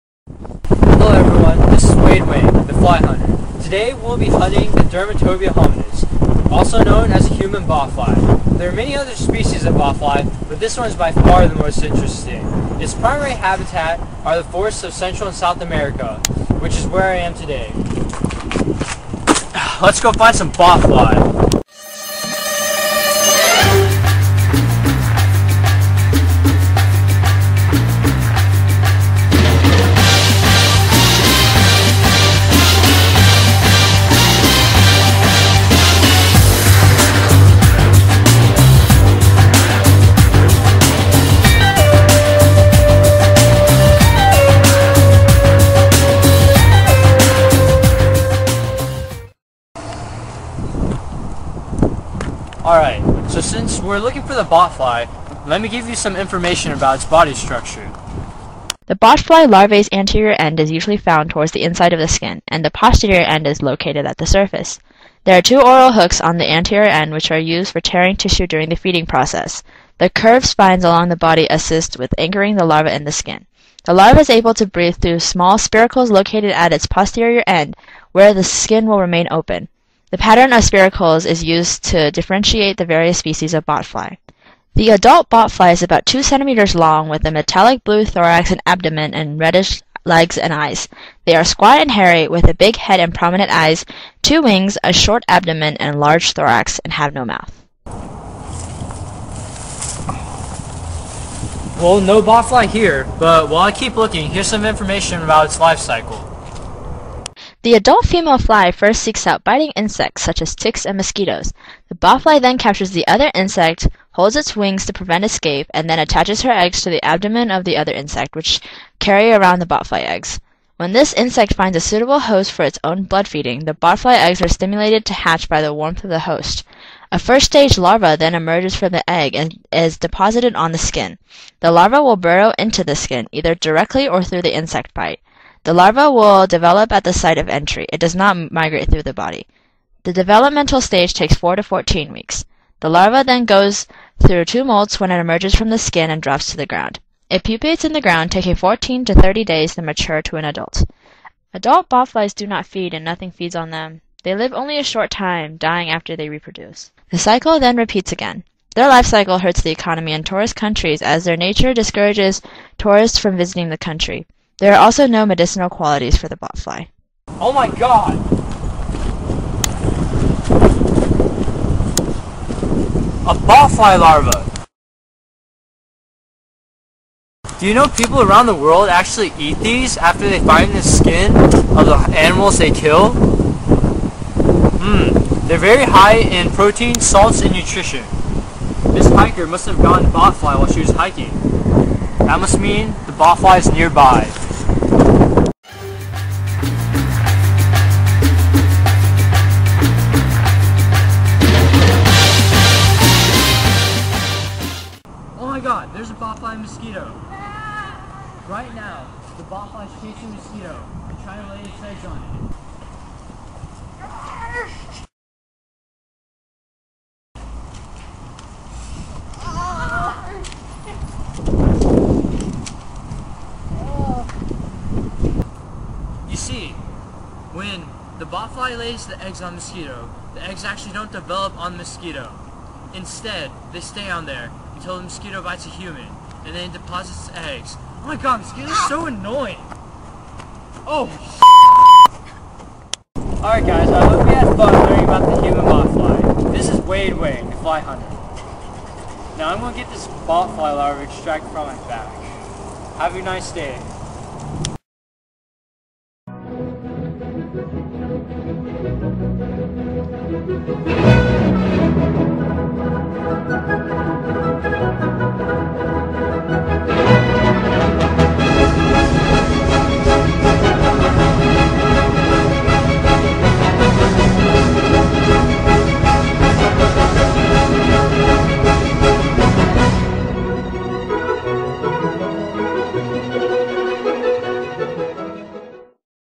The Human Botfly
Wade Wang, the Fly Hunter, explores the details of the fascinating botfly, Dermatoba hominis.

Biolab Feb 25, 2009

Wade Wang: Fly Hunter
Katheryn Shi: Mosquito, dead hiker
Mallory Gunnels: Botfly, cameraperson
Jenny Huang: Voiceover, secondary cameraperson, editor/subber